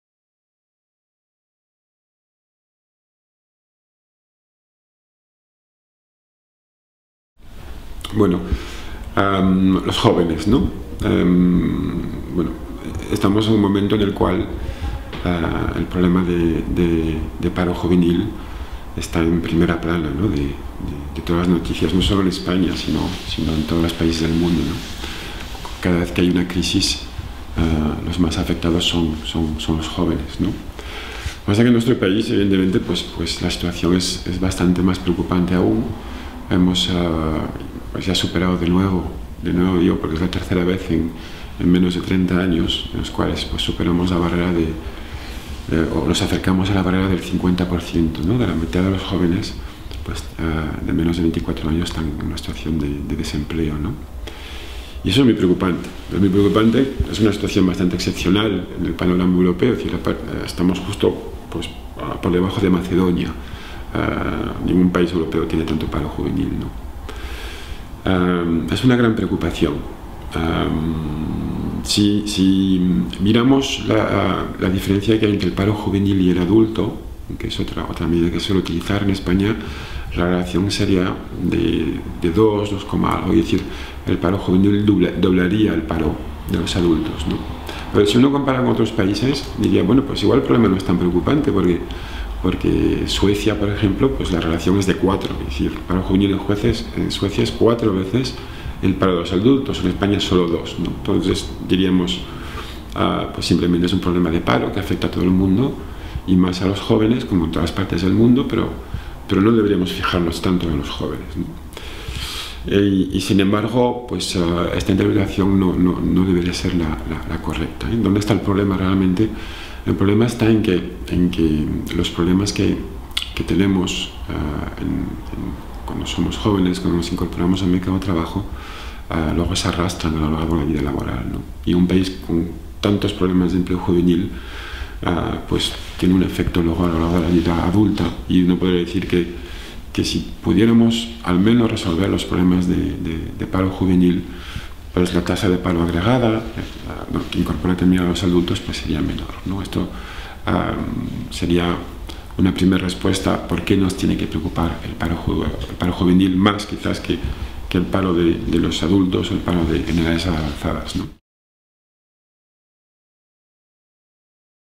Los Jóvenes y la crisis
07/03/2012 - Cada vez que hay una crisis los jóvenes son los más afectados. Por tercera vez en menos de 30 años la mitad de los jóvenes menores de 24 años están en una situación de desempleo.